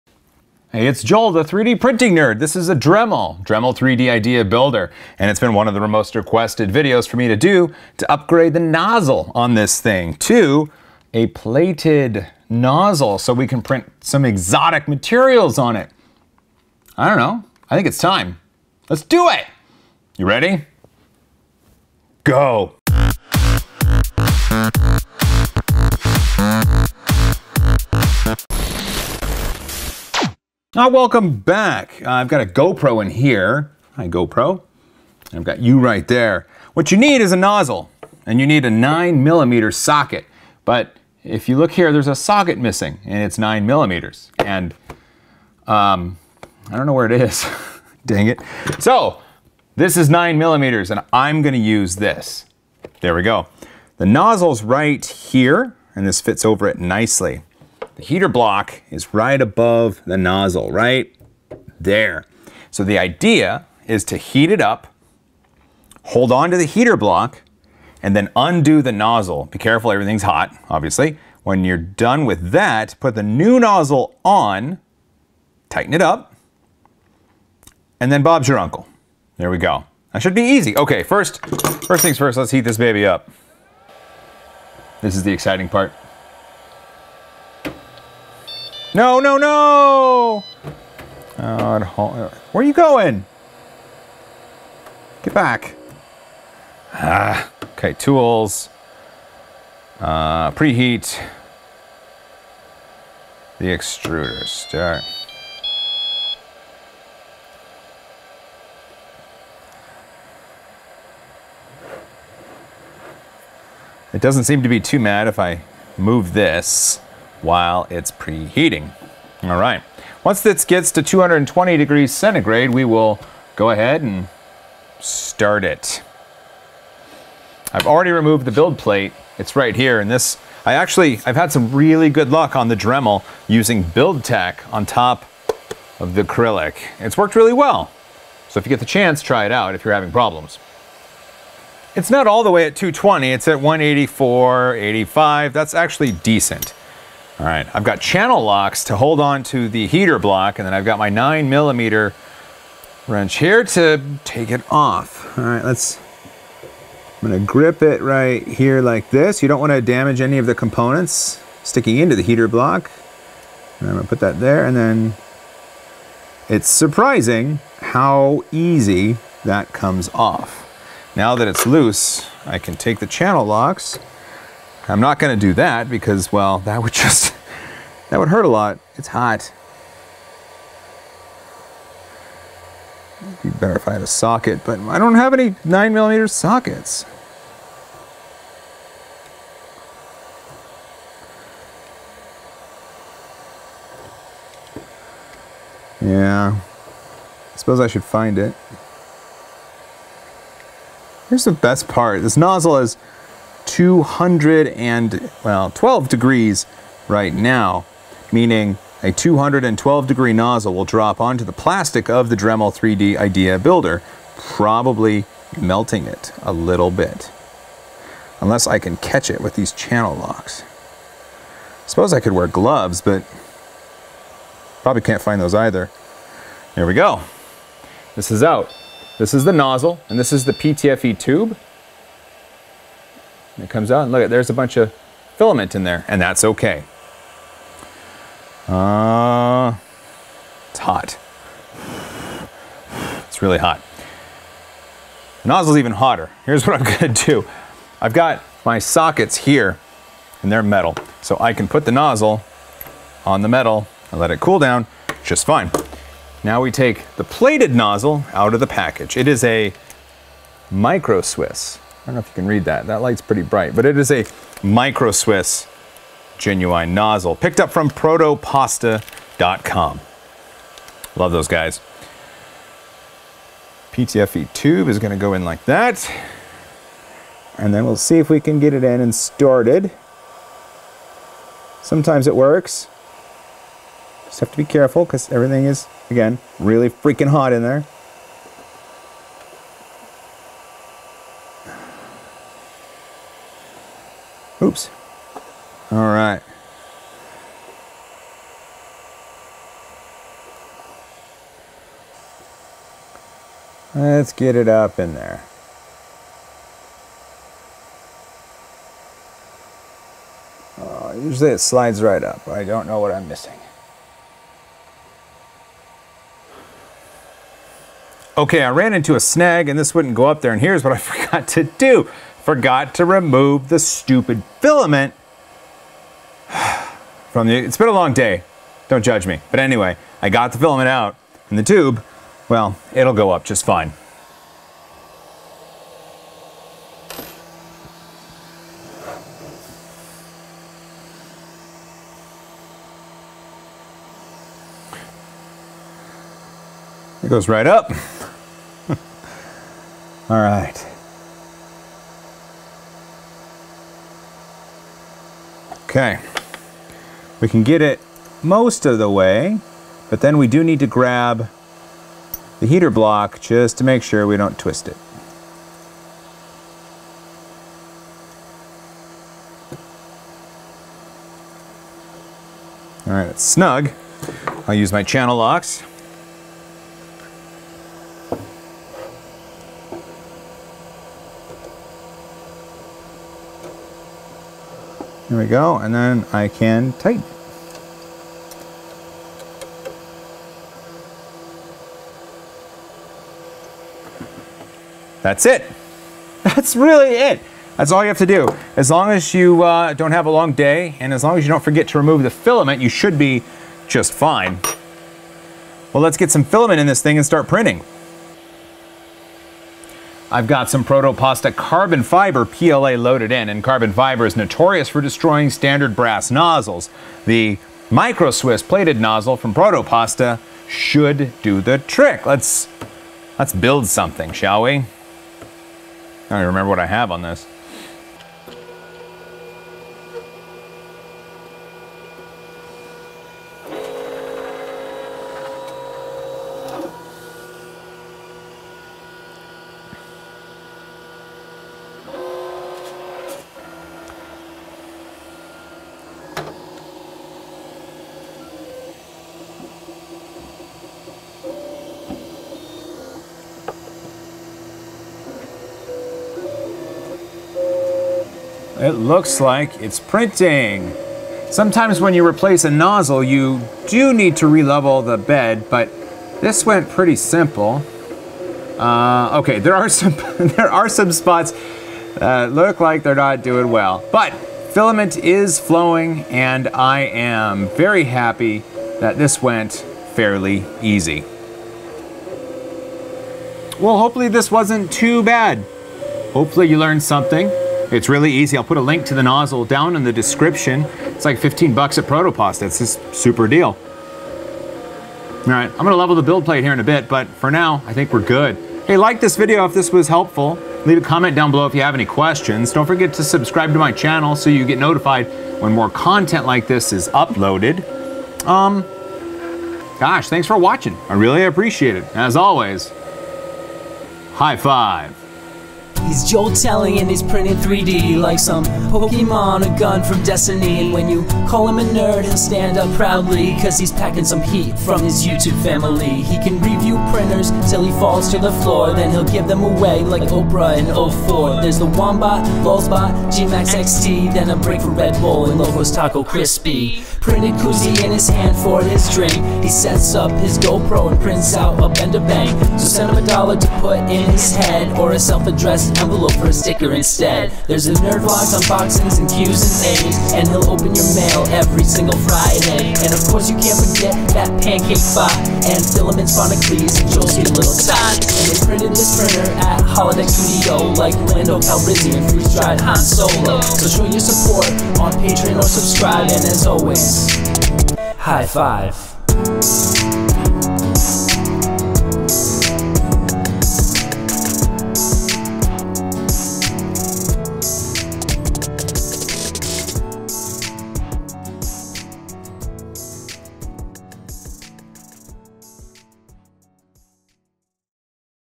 Upgrade the Nozzle on the Dremel 3D Idea Builder
The Dremel 3D Idea Builder is a great printer, and has grown on me more and more since first getting it. I wanted to put a hardened / plated nozzle on it to print exotic materials, and I thought why not film it? Here is how I did it, and how you can do it as well.

▼ HOLY COW BUY A 3D PRINTING NERD SHIRT! ▼